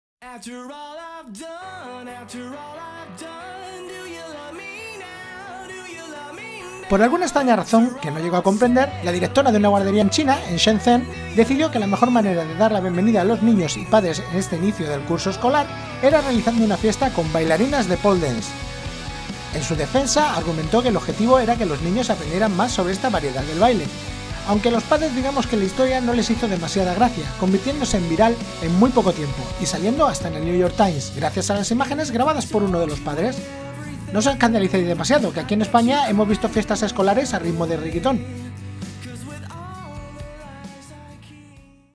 Guardería China la lia parda dando inicio al curso escolar con Bailarinas de Pole Dance
Por alguna extraña razón que no llego a comprender, la directora de una guardería en China en Shenzhen decidió que la mejor manera de dar la bienvenida a los niños y padres en este inicio del curso escolar, era realizando una fiesta con bailarinas de Pole Dance.